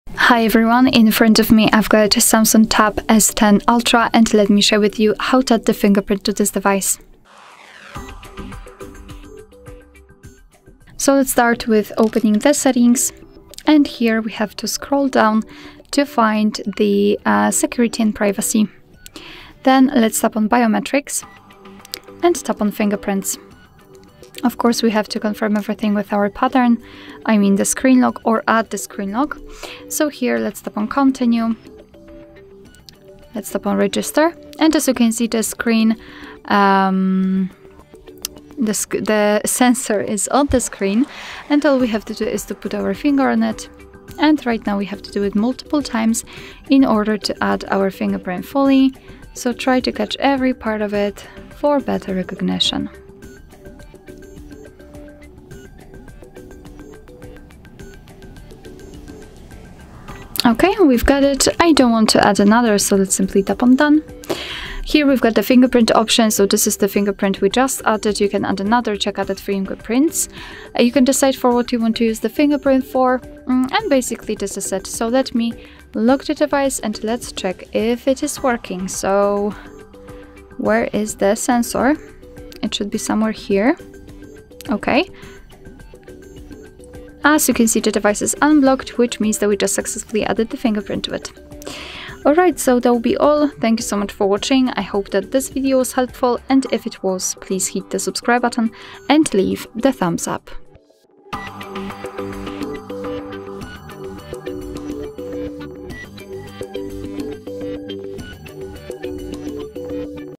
How to Add Fingerprint on Samsung Galaxy Tab S10 Ultra 5G | Enhance Device Security
In this tutorial, we’ll show you how to add a fingerprint to your Samsung Galaxy Tab S10 Ultra 5G, enhancing your device's security and making it easier to unlock your tablet. Fingerprint recognition provides a quick and secure way to access your device, ensuring that your personal information stays protected. We’ll provide step-by-step instructions to guide you through the process of setting up your fingerprint, allowing you to enjoy a seamless and secure user experience.

How to add a fingerprint on Samsung Galaxy Tab S10 Ultra 5G?
What are the benefits of using fingerprint security for your device?
How to manage and delete fingerprints for better security?